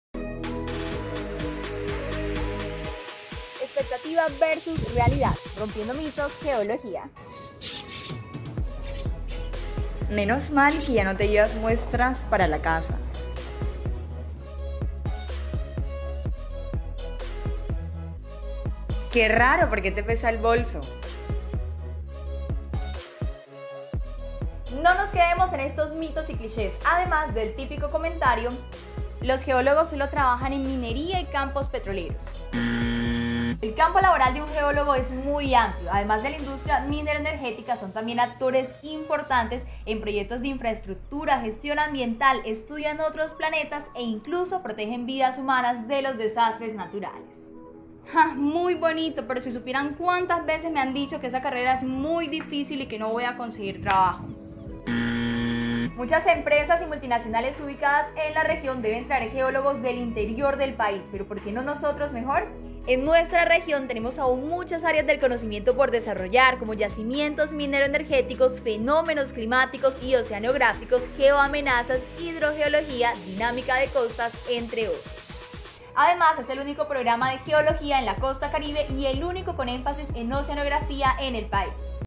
Mito de Geología - Uninorte
¡Aquí te desmentimos los mitos sobre la carrera de Geología!

¿Quieres conocer más? Ingresa aquí y resuelve tus dudas, agendando una asesoría con el coordinador del programa: h